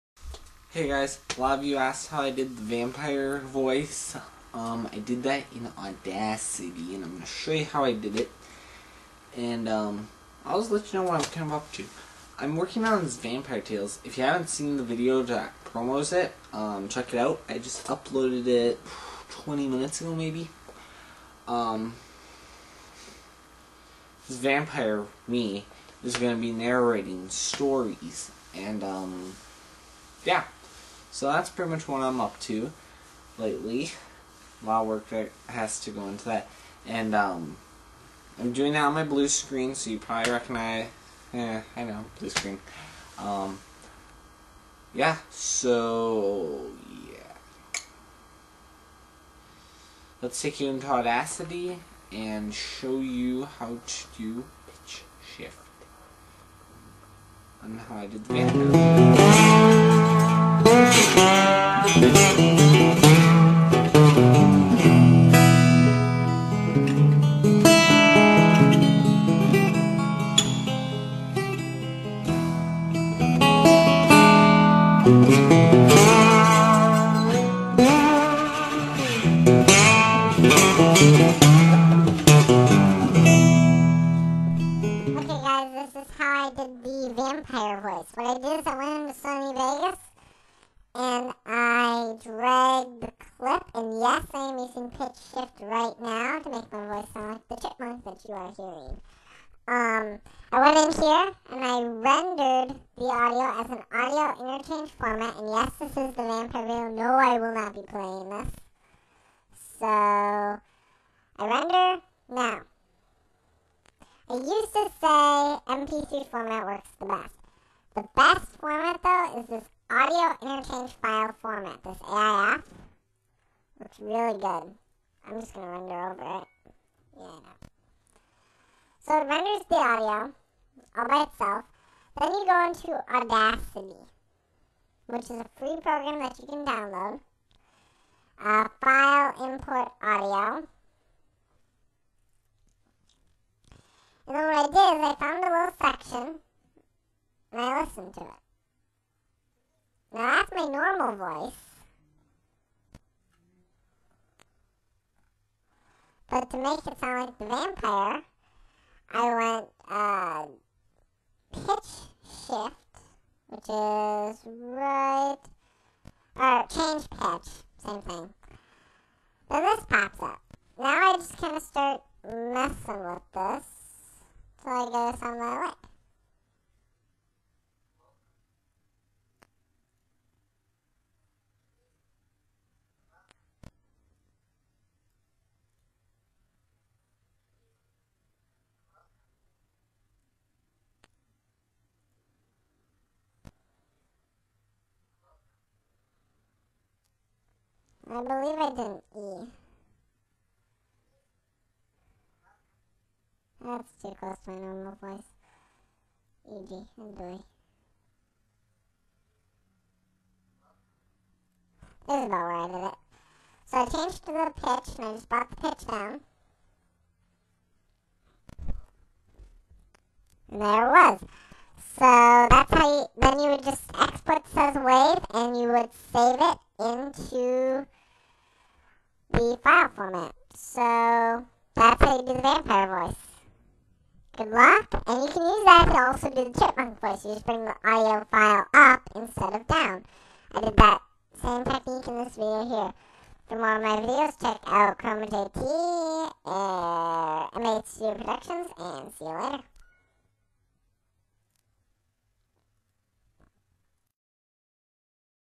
Pitch Shift
Hear I will show you how i made my voice deeper in the vampire tales video